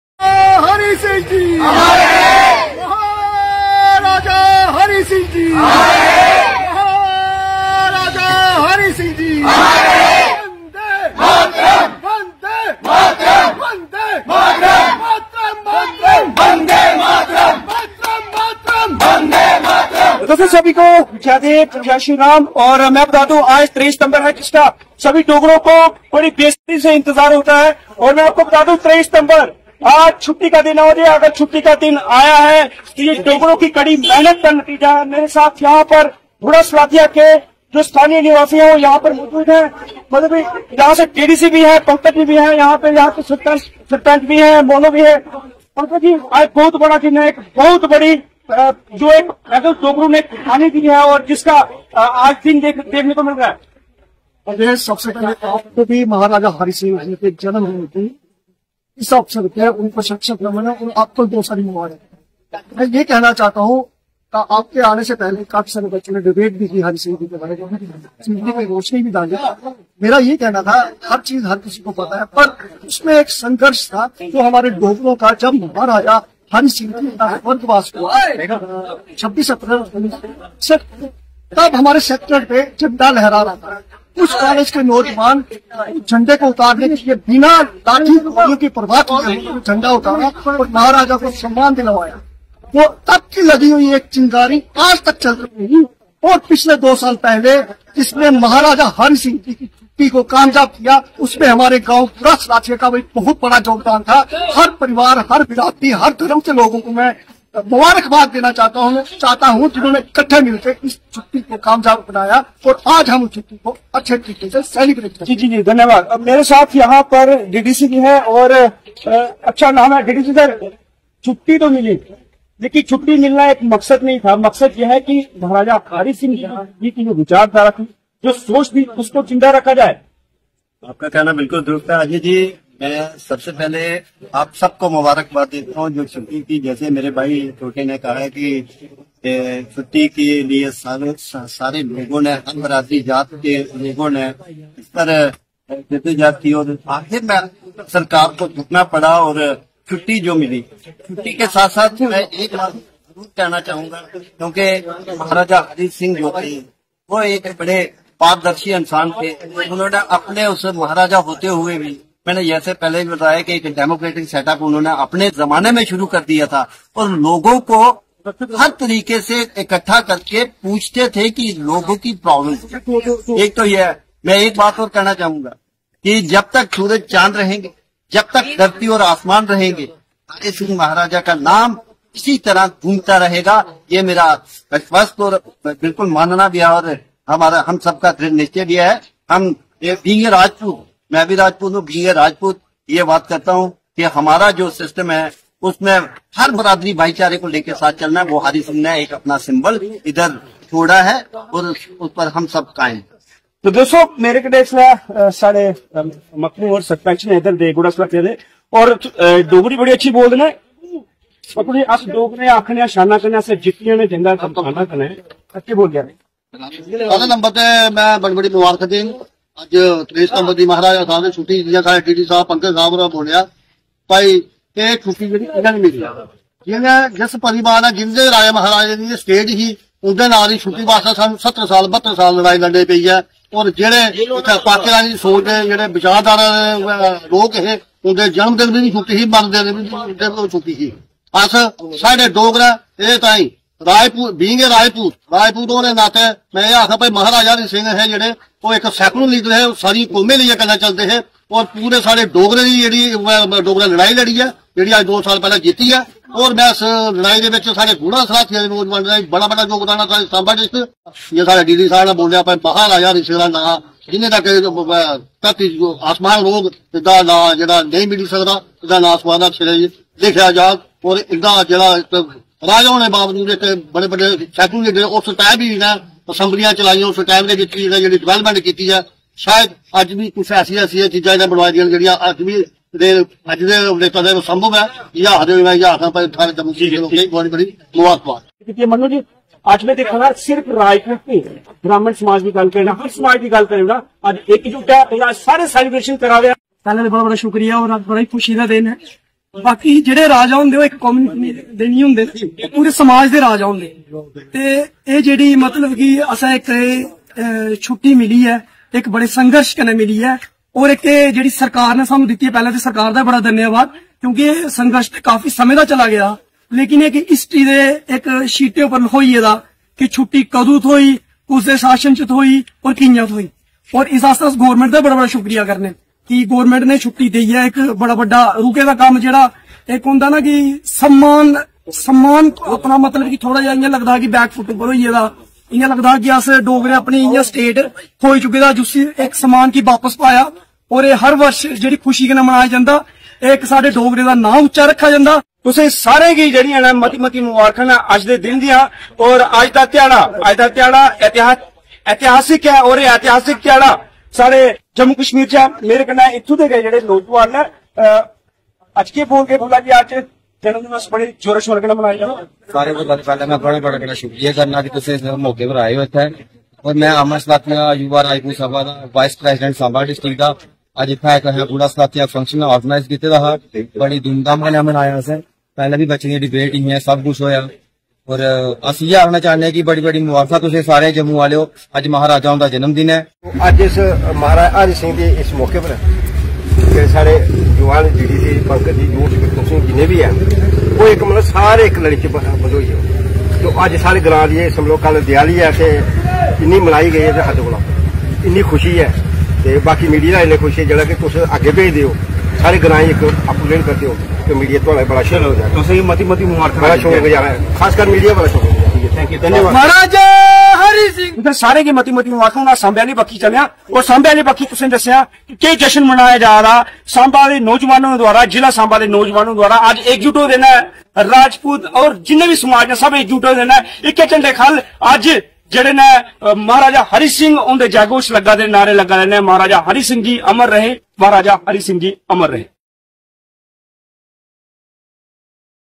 महाराजा हरि सिंह जी अमर रहें, आज उनका जन्मदीवस बड़ी धूम धाम से मनाया गया जम्मू कश्मीर में।
महाराजा हरि सिंह जी अमर रहें, आज उनका जन्मदीवस बड़ी धूम धाम से मनाया गया जम्मू कश्मीर में। गुड़ा सलाथिया में भी नोजबानो में काफी उत्साह देखने को मिला।